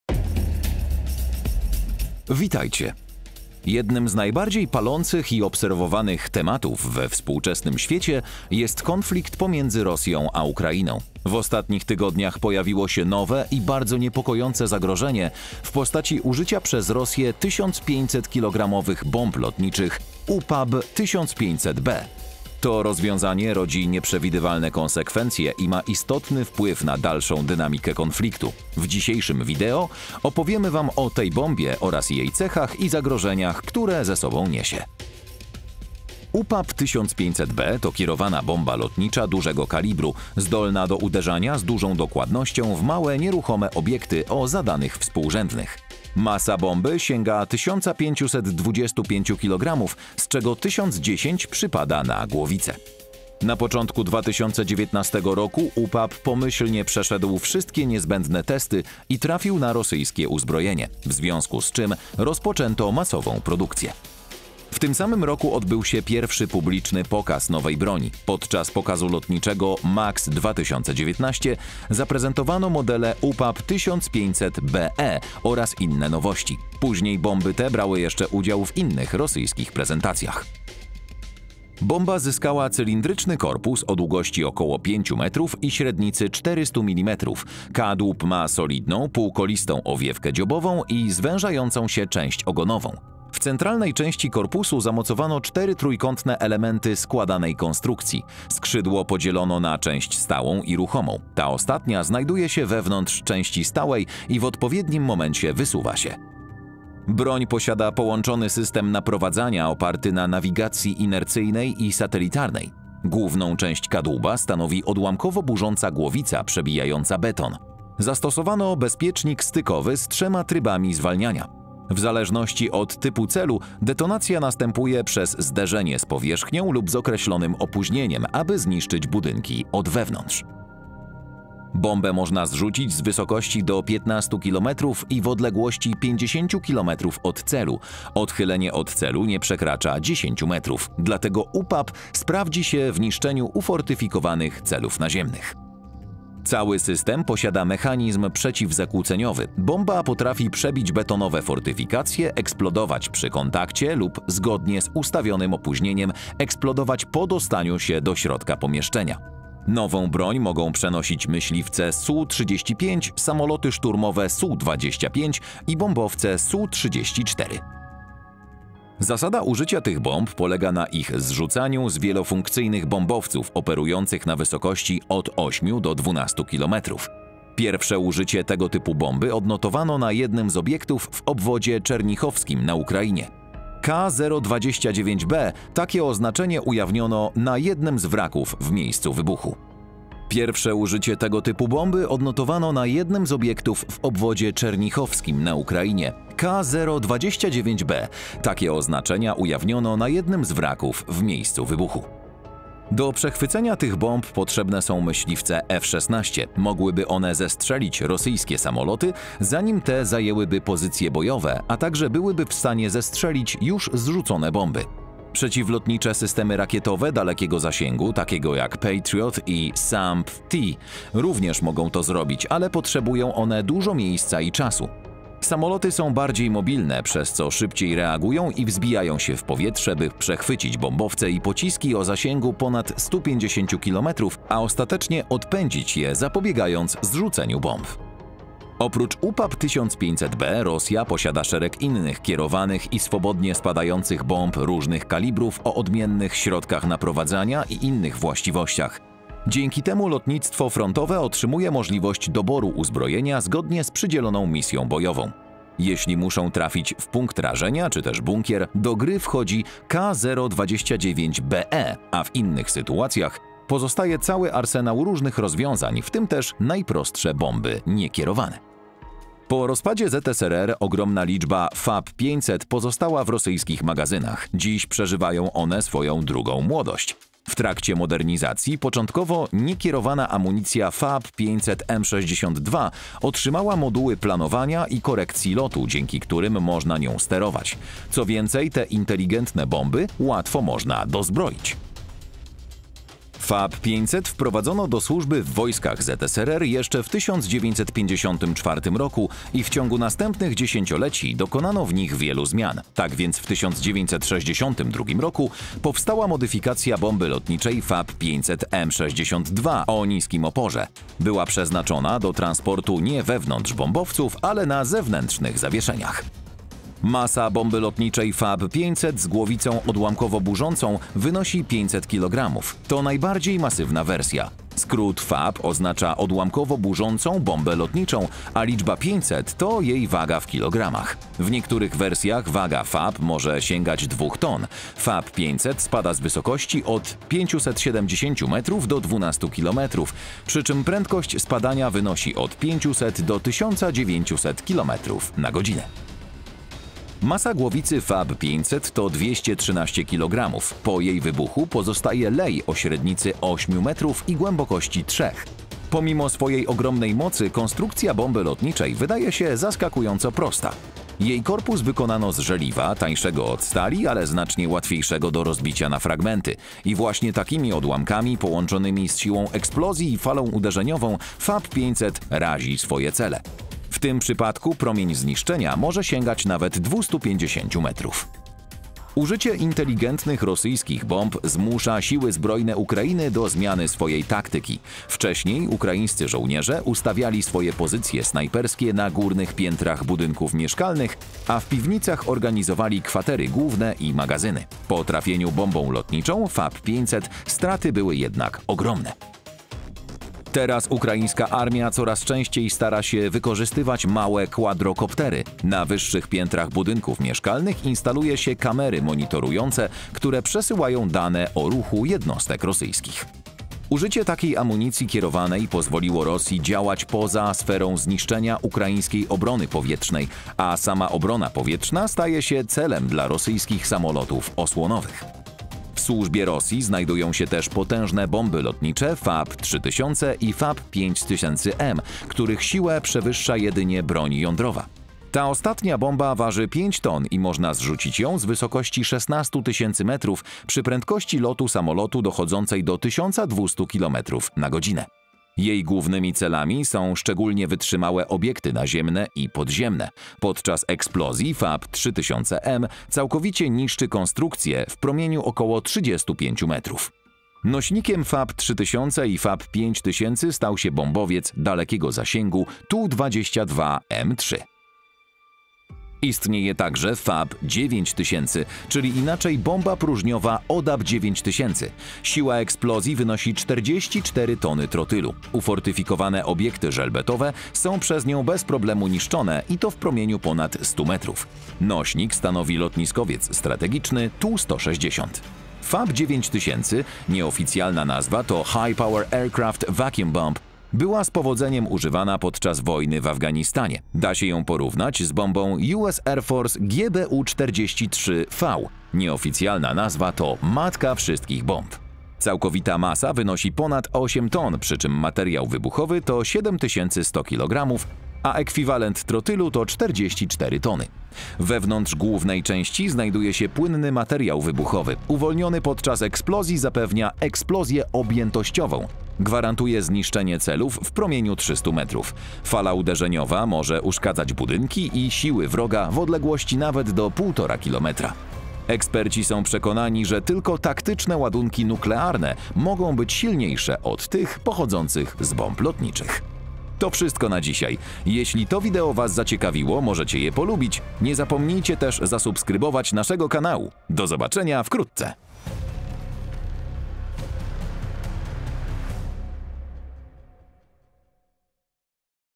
Nowe rosyjskie bomby lotnicze stanowią zagrożenie nie tylko dla Ukrainy
Jednym z najbardziej palących i obserwowanych tematów we współczesnym świecie jest konflikt między Rosją a Ukrainą. W ostatnich tygodniach pojawiło się nowe i bardzo niepokojące zagrożenie w postaci użycia przez Rosję 1500-kilogramowych bomb lotniczych UPAB-1500B. To rozwiązanie rodzi nieprzewidywalne konsekwencje i ma istotny wpływ na dalszą dynamikę konfliktu. W dzisiejszym video opowiemy Wam o tej bombie, jej cechach i zagrożeniach, które ze sobą niesie.